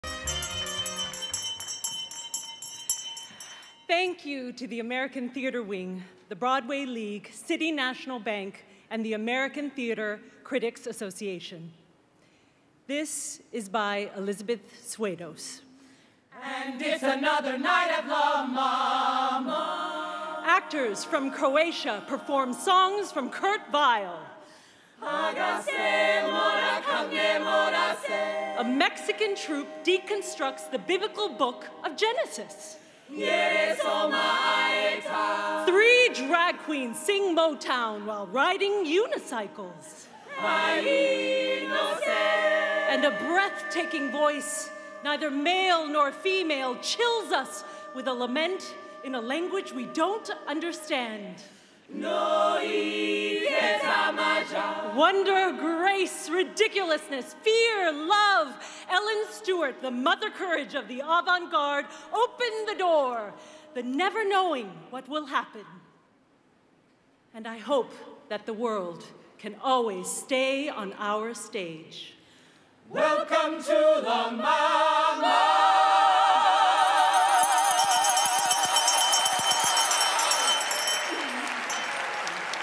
Acceptance Speech: La MaMa E.T.C. (2018)
La MaMa E.T.C. accepts the Regional Theatre Award during the Creative Arts Awards portion of the 2018 Tony Awards ceremony.